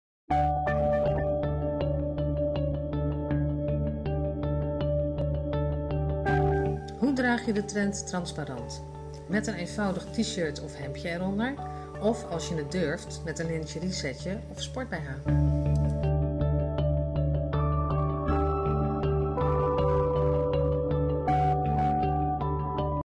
Durf jij transparant?
Spannend, speels en een tikkeltje gewaagd. Maar hoe draag je het?
 Nou, zo! Kijk in Vriendin 22 waar je deze kleding shopt!

UPLOAD SCHEMA
Elke dinsdag en donderdag een nieuwe video.